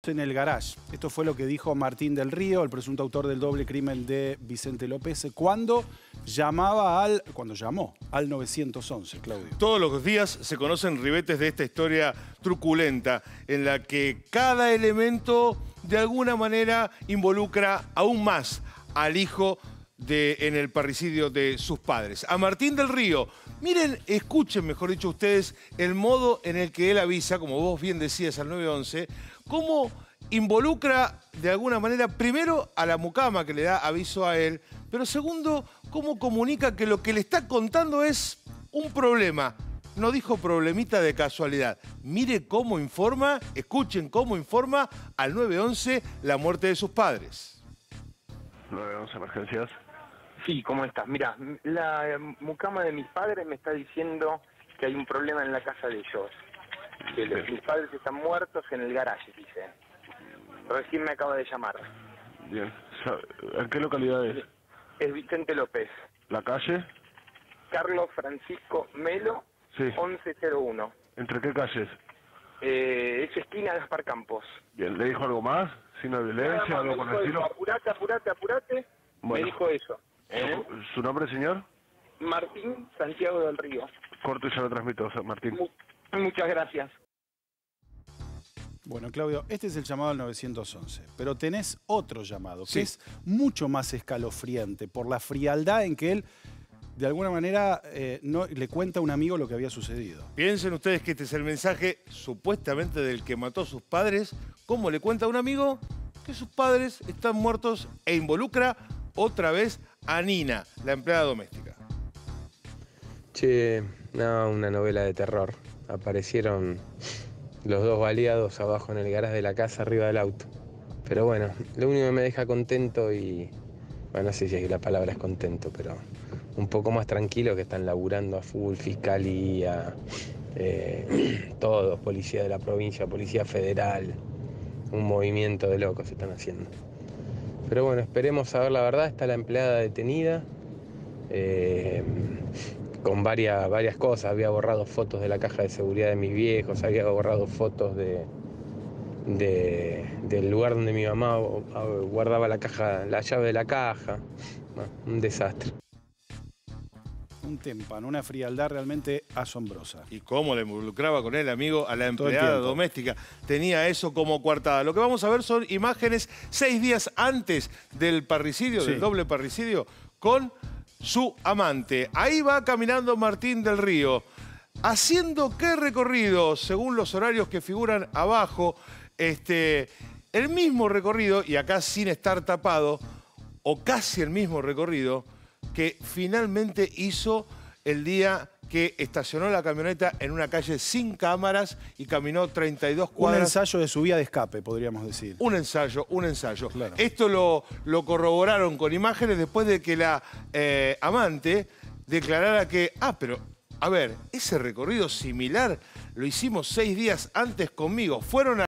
El audio de Martín Del Río a un amigo tras el doble crimen en Vicente López: "Una novela de terror"
Tras una nueva semana donde se van conociendo más detalles sobre la causa que investiga el doble crimen de Vicente López de José Enrique Del Río (74) y Mercedes Alonso (72), se difundieron los audios del 911 que realizó el único detenido Martín Del Río, en mensaje que le envió a un amigo y un video donde se lo puede hacer un recorrido sospechoso con su amante.